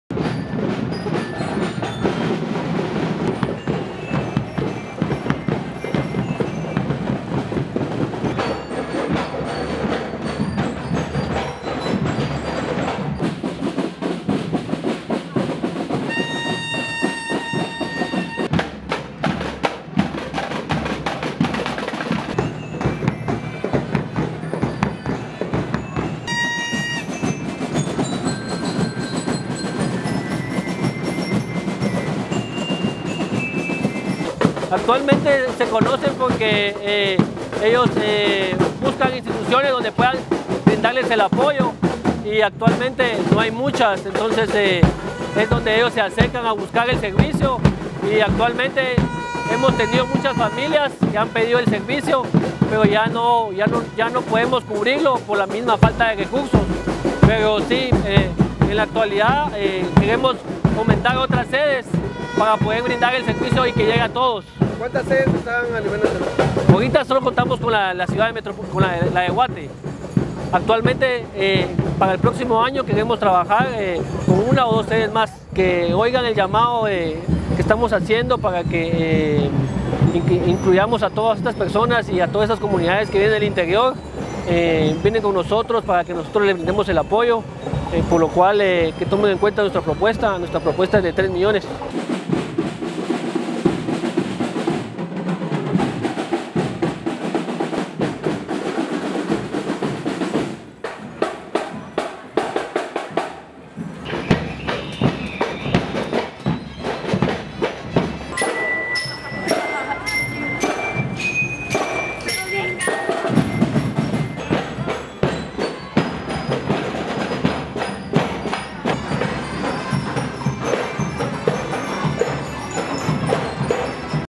Infantes y adultos marchan en conmemoración del Día Internacional de la Sordera
POR: NOÉ PEREZ-DCATV
En el marco de la conmemoración del Día Internacional de las Personas Sordas o Día Internacional de la Sordera, la Asociación Nacional de Sordos de Guatemala (Ansogua) realizó hoy una marcha que recorrió el Paseo la Sexta, en la zona 1 de la ciudad de Guatemala.
El festejo, armonizado por bandas escolares, fue acompañado por niños, niñas y adultos con esta discapacidad.
Edgar Mori Díaz, administrador de la Ansagua, dijo al respecto que “además de celebrar esta fecha, buscamos ser incluyentes con las personas no oyentes”.
“…conforme a la población del país, se estima que el 1.4 por ciento padece sordera”, expresó Mori, quien agregó que la Asociación se sostiene gracias a un subsidio brindado por el Ministerio de Salud Pública y Asistencia Social.
De tal cuenta, el administrador aseguró que el reto es "habilitar más sedes a nivel nacional, ya que la demanda de apoyo por parte de personas no oyentes es creciente”.
Todas las personas que acuden a la entidad se les verifica si tiene “frecuencia en cada oído; de ser así, reciben terapias de habla y lenguaje. En el caso de las personas totalmente sordas reciben una terapia de lenguaje de señas”, añadió Mori.
Ansgua también brinda atención psicológica para las niñas y niños que han padecido maltrato y violencia: “Todos los servicios son totalmente gratis”, concluyó el entrevistado.